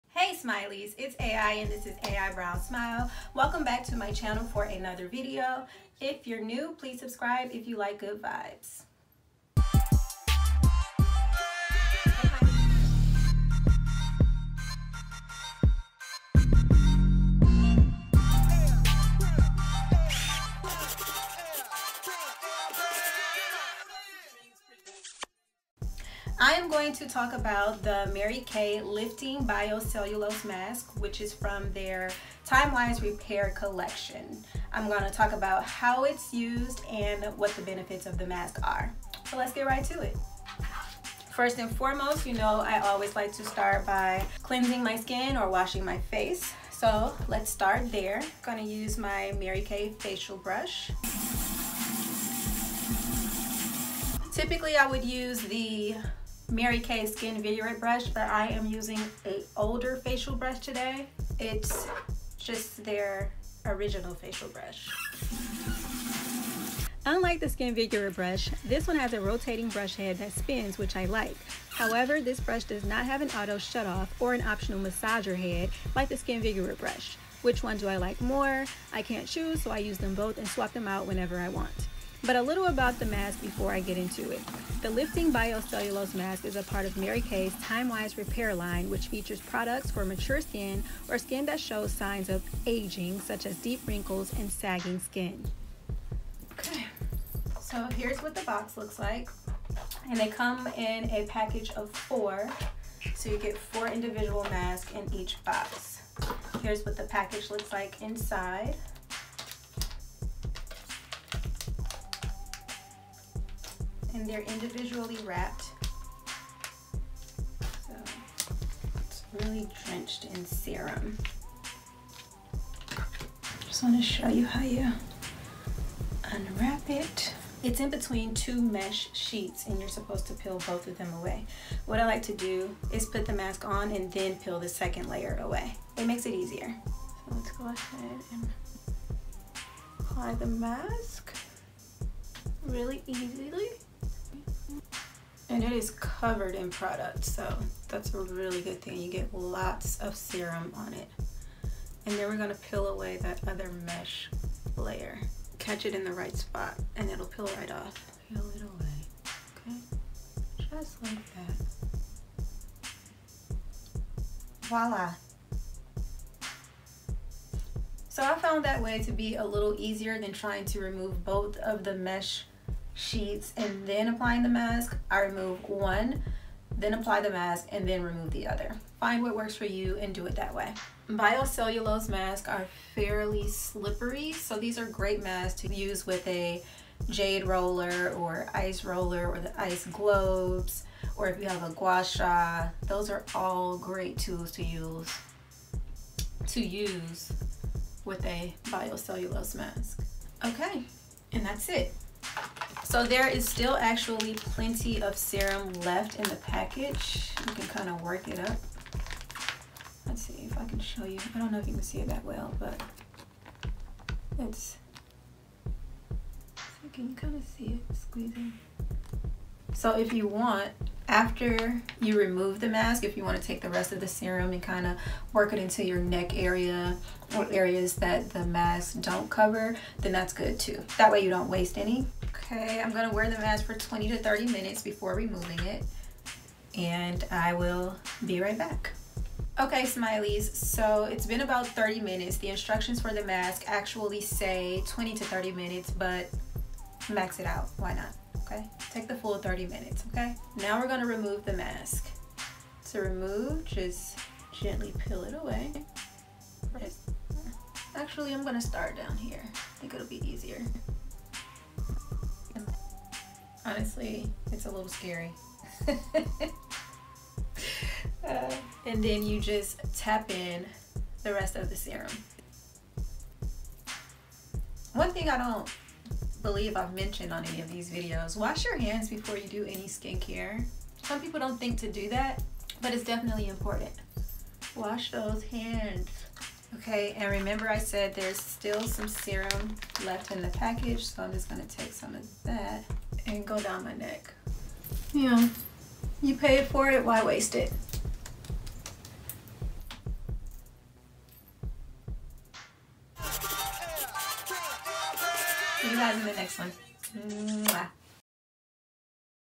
aibrownsmile | K-Beauty Inspired Face Mask: TimeWise Repair Lifting Bio-cellulose Mask by Mary Kay
Hey Smileys hey!! Bio-cellulose is a thin, plant-based material derived from coconut water. The lifting bio-cellulose face mask from Mary Kay's TimeWise Repair line combats dull skin and wrinkles. Skin is visibly more radiant, soft and hydrated after a single use! Take 20-30 minutes to treat yourself to some much-deserved pampering.

Glowing skin is always in!

XoXo,
AI

Spread L O V E.
Promote P E A C E.
Cultivate P O S I T I V I T Y.
Find the B E A U T Y in all things.

smil·ey
/ˈsmīlē/

@Hustle Dream Production